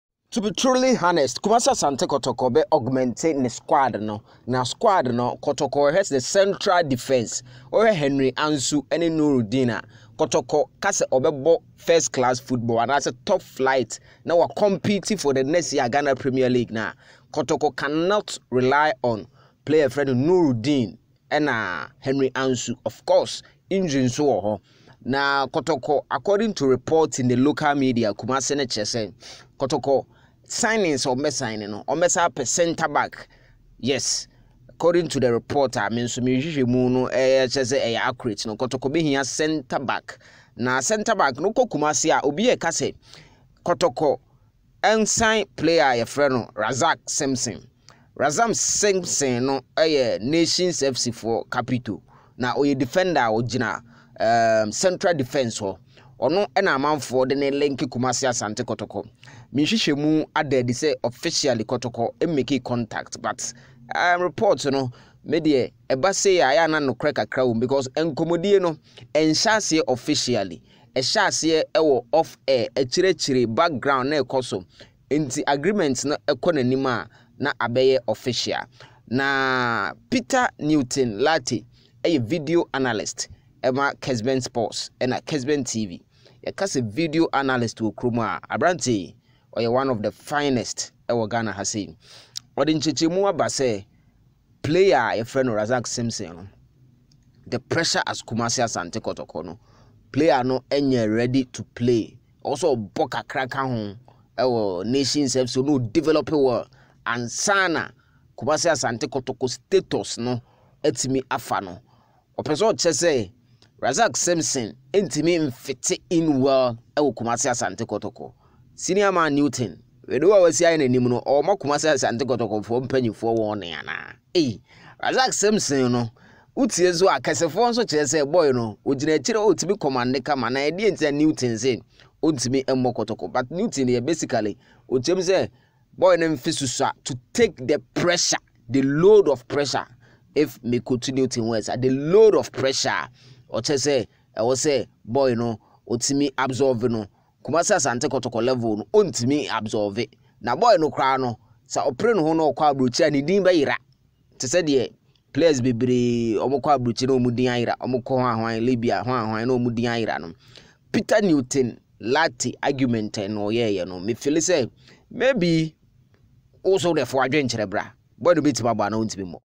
RAZAK SIMPSON IS NOT RIPE TO PLAY KOTOKO - NEWTON LARTEY REVEALS WHY
Newton has warned Kotoko about some signing. watch video for details.